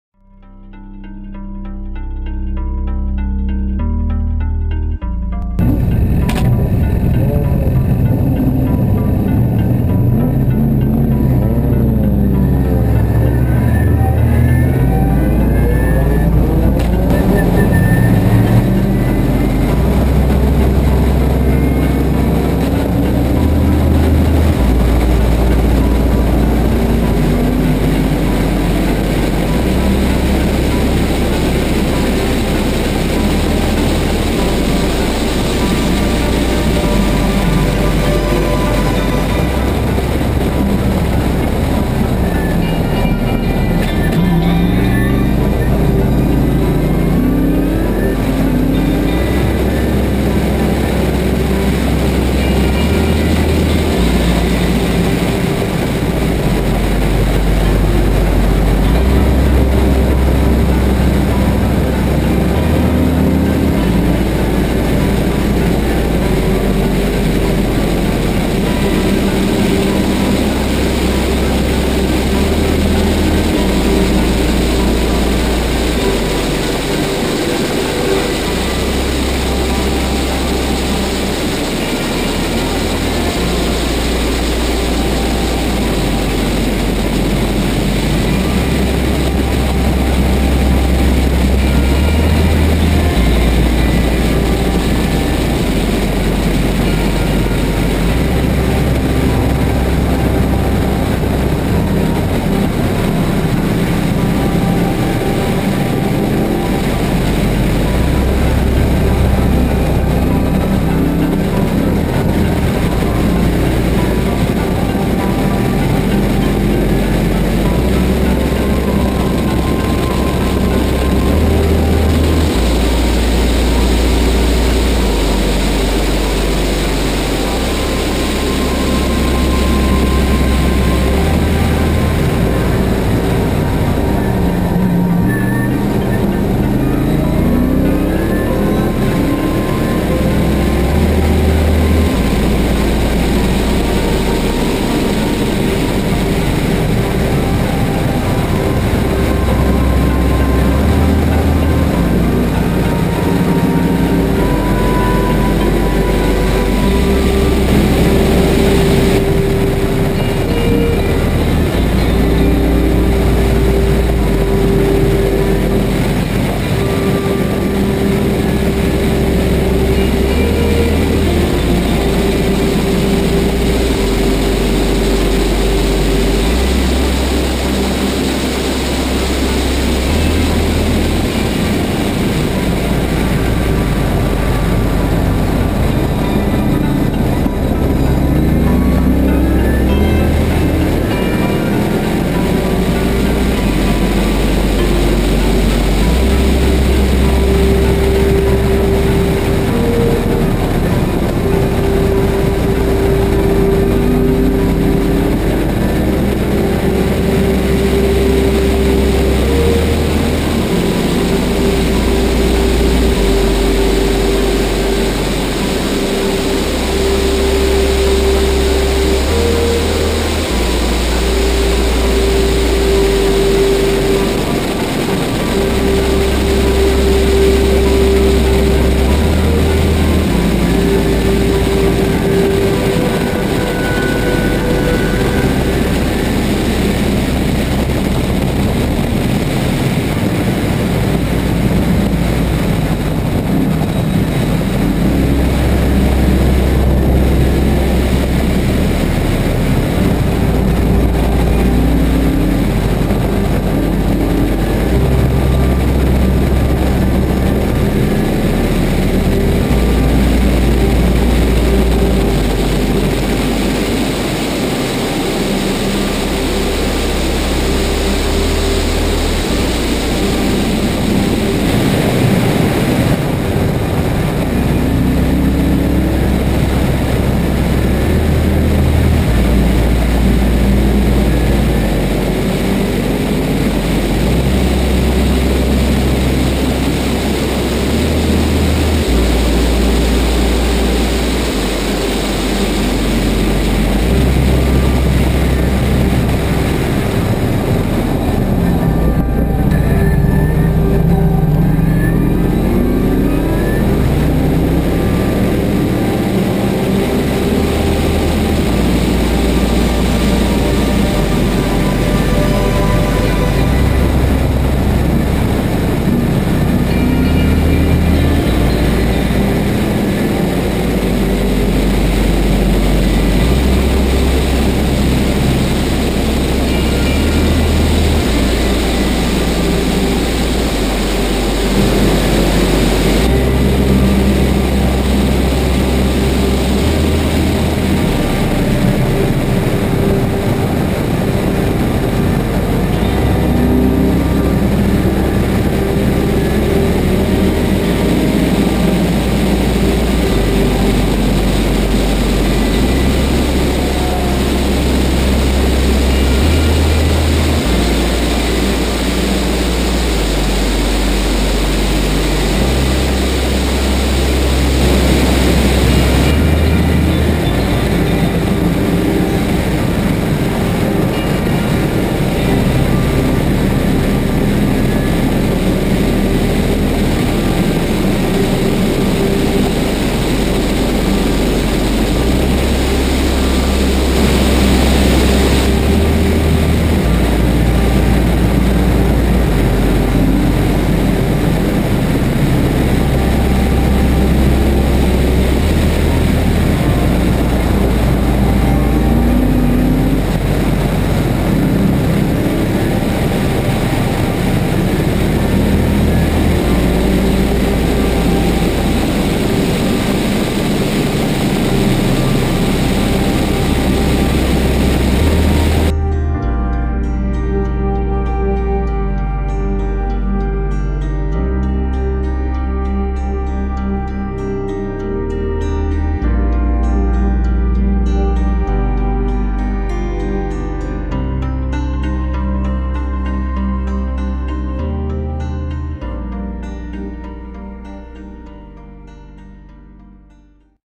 CN-Cobra RS4 im Histo Cup am Salzburgring 9/2018
Rennen 1 im Histo Cup Klasse STW mit der CN-Cobra RS4 am Salzburgring 9/2018.